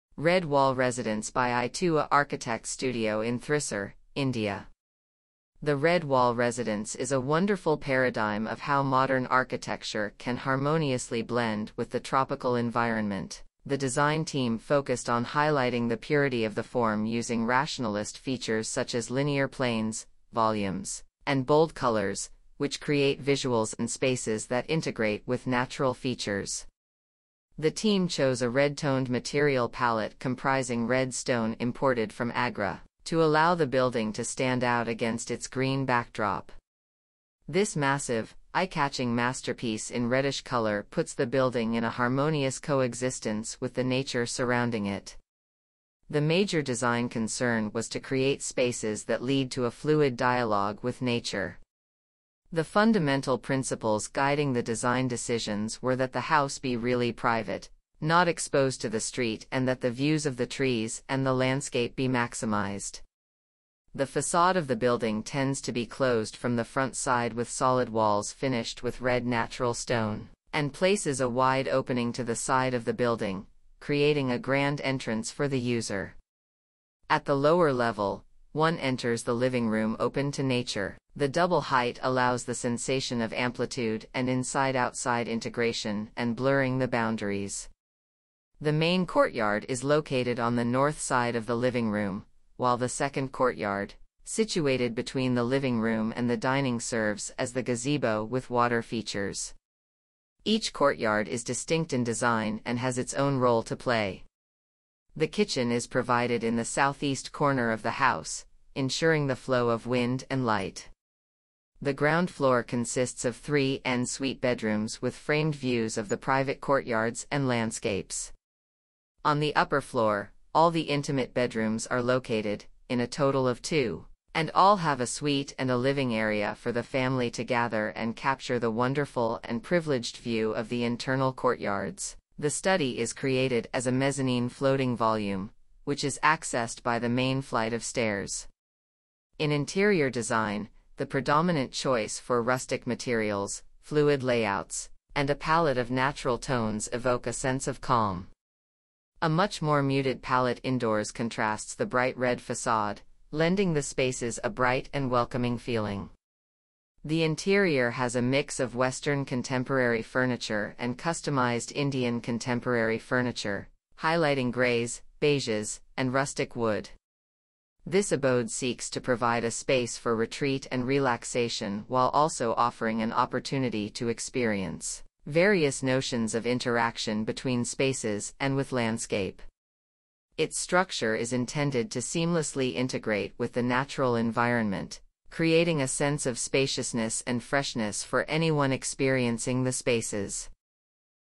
Red Wall Residence By i2a Architects Studio In THRISSUR, INDIA
The Red Wall Residence is a wonderful paradigm of how modern architecture can harmoniously blend with the tropical environment. The design team focused on highlighting the purity of the form using rationalist features such as linear planes, volumes, and bold colors, which create visuals and spaces that integrate with natural features. The team chose a red-toned material palette comprising red stone imported from Agra to allow the building to stand out against its green backdrop. This massive, eye-catching masterpiece in reddish color puts the building in a harmonious coexistence with the nature surrounding it.

Archi Studio

Architects: i2a Architects Studio
Area:
6000 ft²
Year:
2023
Photographs:Running Studios
Manufacturers:  Saint-Gobain, Ardex endura, Asian Paints, Bosch, Europa, Exteria, Greenlam, Greenply, Hettich, Jaquar, Kohler, Tile Vetrified Tile
Interior Execution: KUDIL INTERIOR
Principal Architect: Manuraj C R
Project Architect: Amal Suresh
Furniture Design: Yellow Petals
Main Contractor : KUDIL BUILDERS
Joinery: Anila Industries
Interiors – Fabrics: CHIKOOS, EVONNE DECOR
City: Thrissur
Country: India